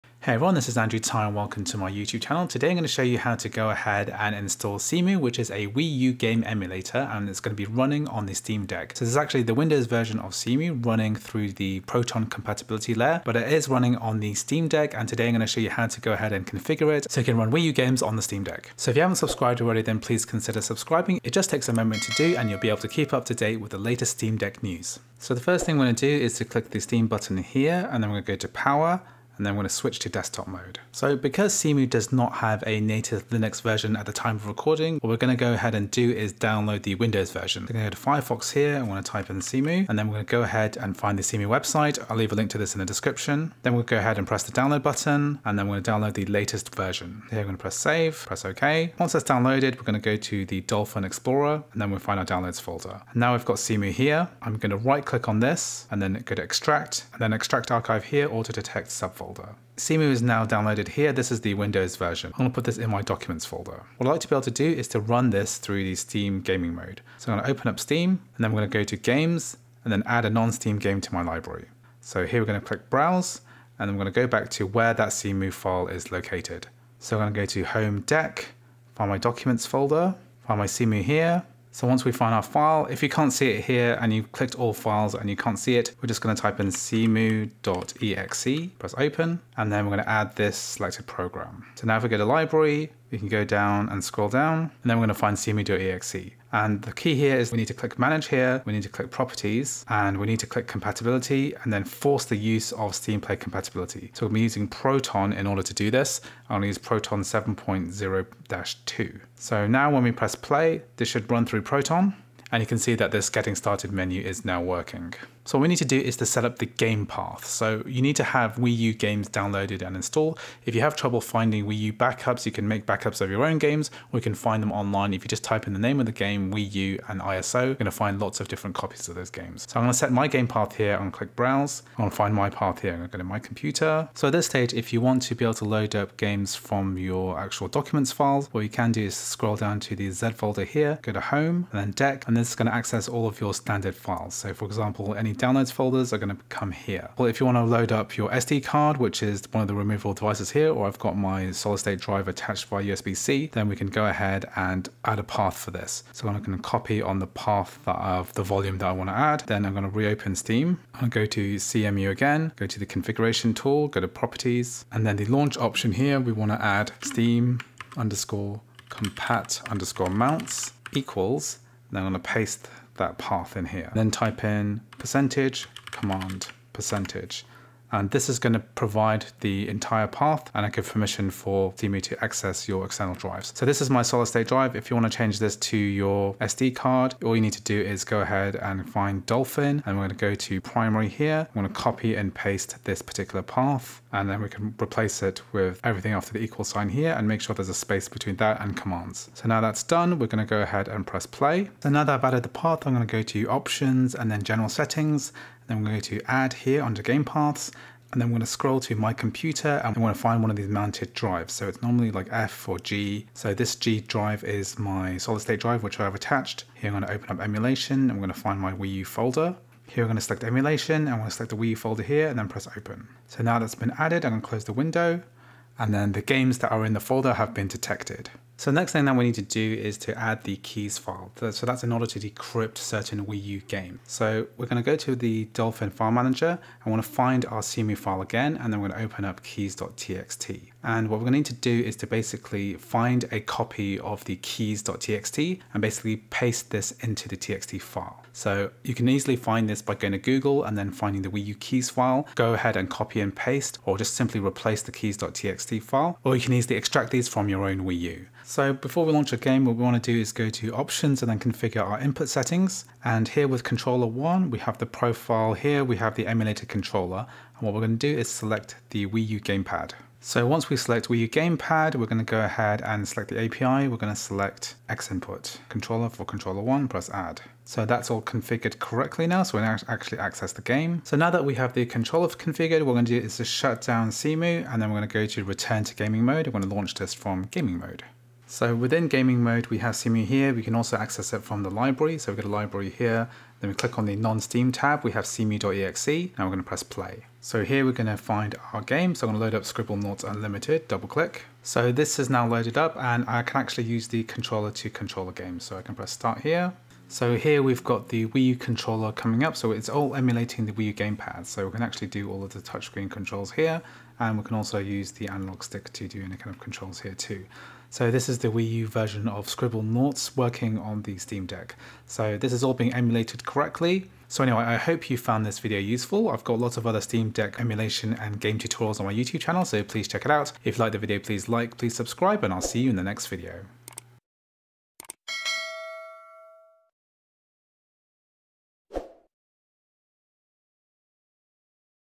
How To Emulate Wii U Games Steam Deck - Cemu Setup Tutorial (Proton Method)
STEAM_COMPAT_MOUNTS=/run/media/deck/. %commands%

00:00 Download Cemu (Windows)
01:03 Add to Steam
01:55 Download Wii U games
02:09 (Optional) Set game path to SD or external drive
03:21 (Optional) Add game folder
03:28 Add keys file
04:18 Steam Deck controller setup
04:41 Launch Wii U game in Steam Deck Gaming Mode

► My Recording Setup:
►► My equipment: